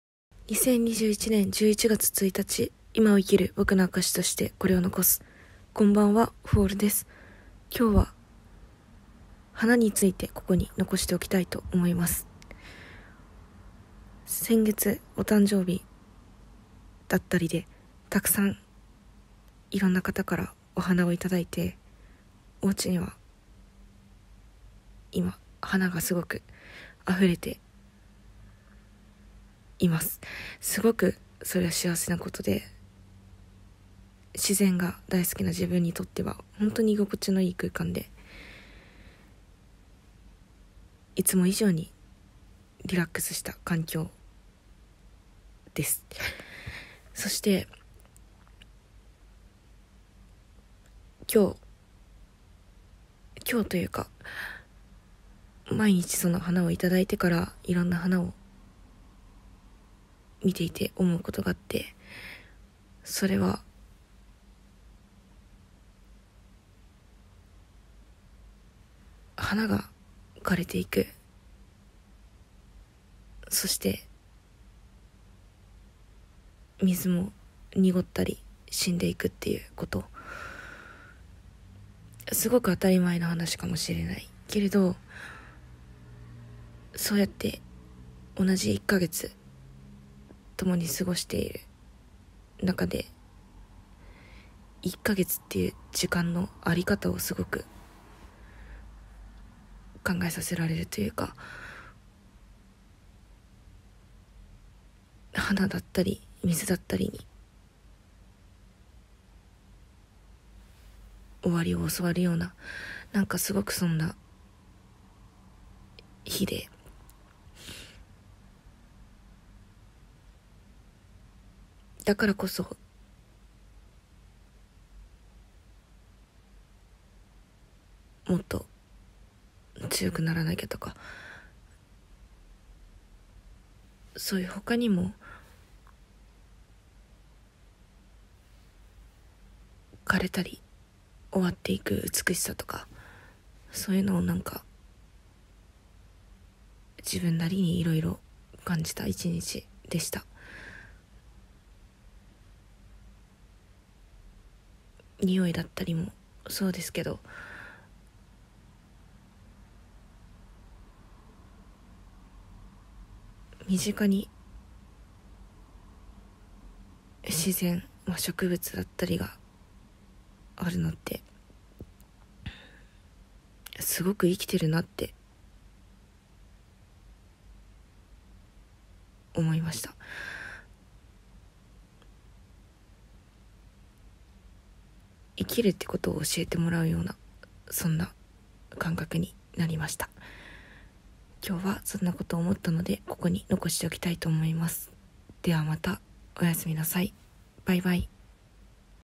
【VLOG】2021.11.1「それが終わりを告げる」
4ȺLL.(フォール)です。
今日は、家に飾っている花について残しておきます。

それが終わりが来ることを告げる。
目を逸らしがちだけれど、
真っ直ぐにそれを受け止めている花たちは
とても美しく儚いと思った。
花のように生きたい。